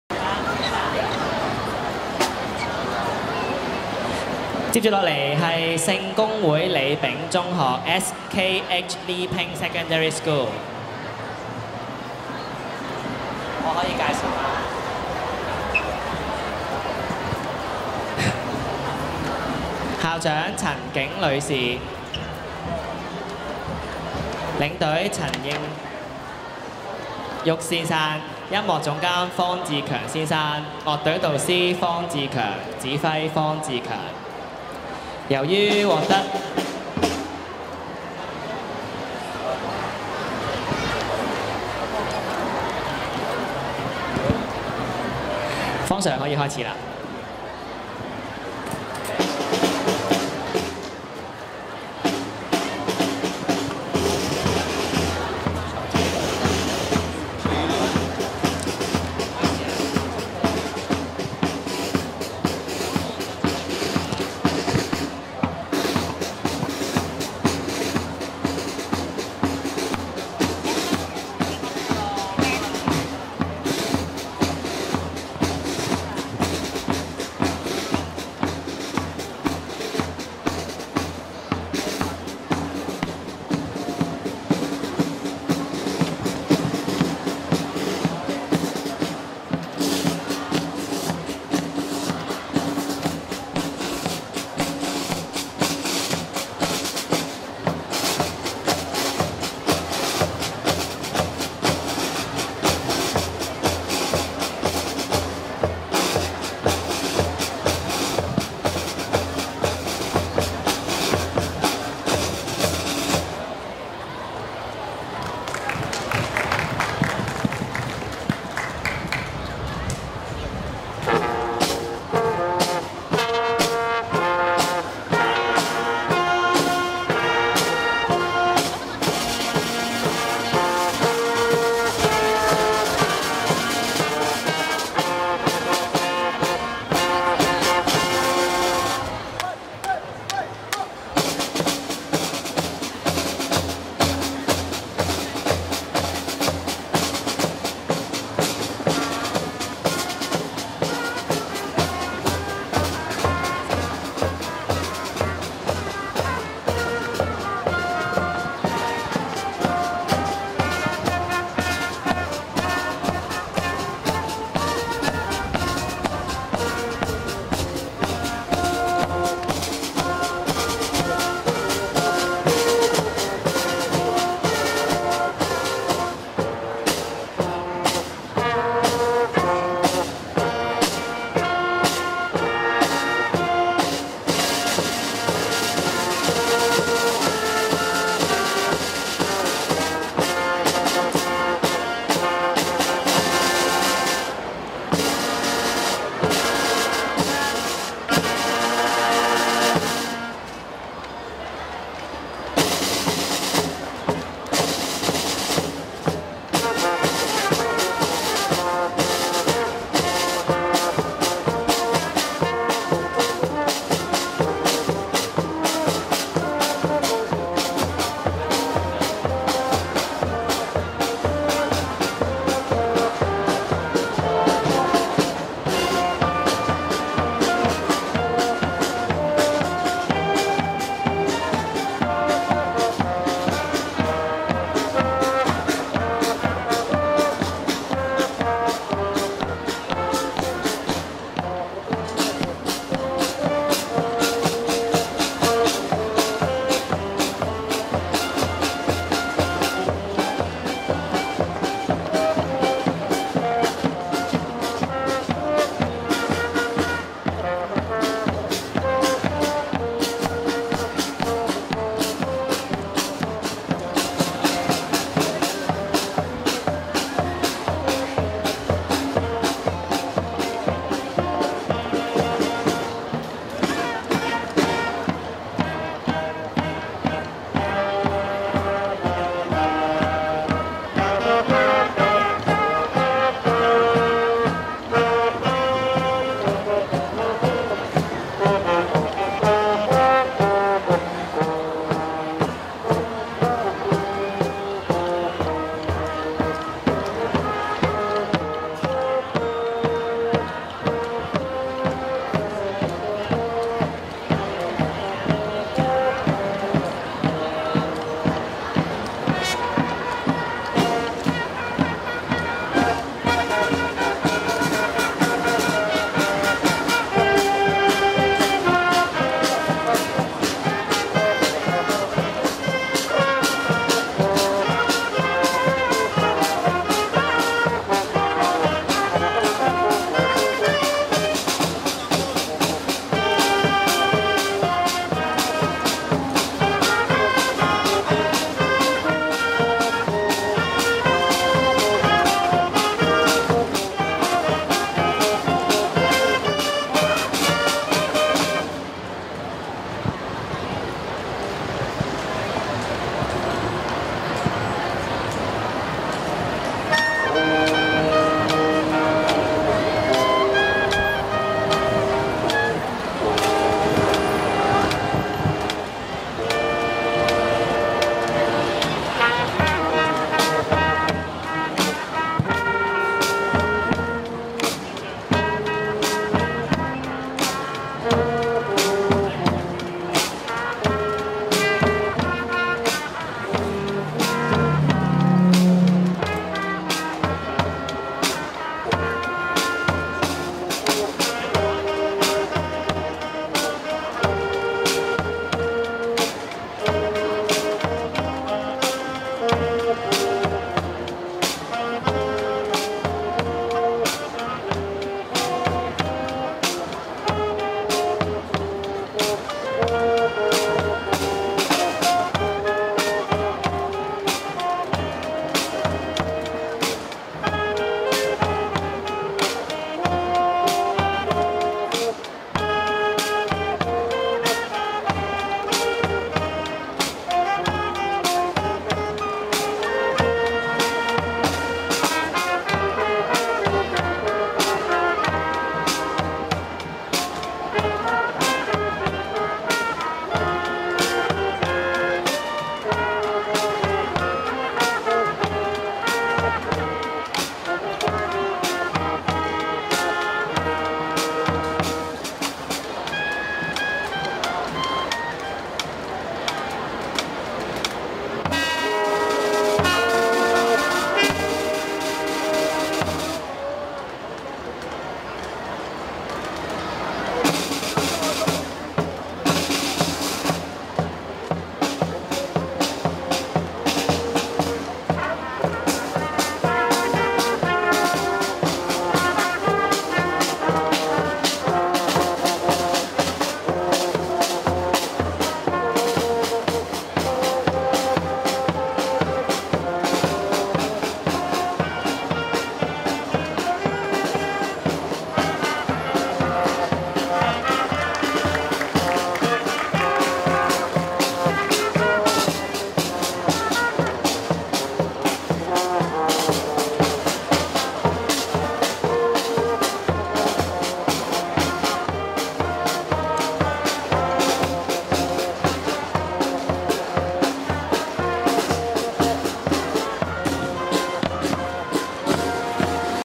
2012 香港國際青年步操樂隊大賽 HKIYMBC - 聖公會李炳中學 S.K.H. Li Ping Secondary School
2012 香港國際青年步操樂隊大賽
步操樂隊比賽 - 本地邀請賽
聖公會李炳中學
2012 Hong Kong International Youth Marching Band Competition
Marching Band Competition - Local Invitational
S.K.H. Li Ping Secondary School

日期 Date: 01 JULY 2012

地點: 荃灣沙咀道遊樂場
Venue: Tsuen Wan Sha Tsui Road Playground